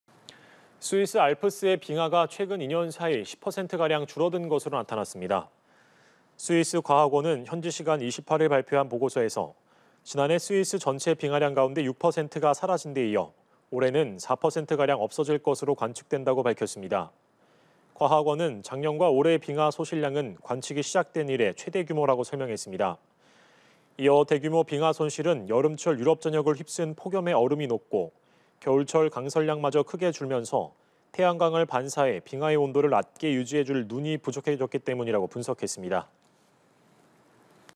"스위스 알프스 빙하, 불과 2년 만에 10% 감소" / 연합뉴스TV (YonhapnewsTV)
"스위스 알프스 빙하, 불과 2년 만에 10% 감소"

스위스 알프스의 빙하가 최근 2년 사이 10% 가량 줄어든 것으로 나타났습니다.

스위스 과학원은 현지시간 28일 발표한 보고서에서 "지난해 스위스 전체 빙하량 가운데 6%가 사라진 데 이어 올해는 4% 가량 없어질 것으로 관측된다"고 밝혔습니다.

과학원은 "작년과 올해 빙하 소실량은 관측이 시작된 이래 최대 규모"라고 설명했습니다.

이어 "대규모 빙하 손실은 여름철 유럽 전역을 휩쓴 폭염에 얼음이 녹고, 겨울철 강설량마저 크게 줄면서 태양광을 반사해 빙하의 온도를 낮게 유지해 줄 눈이 부족해졌기 때문"이라고 분석했습니다.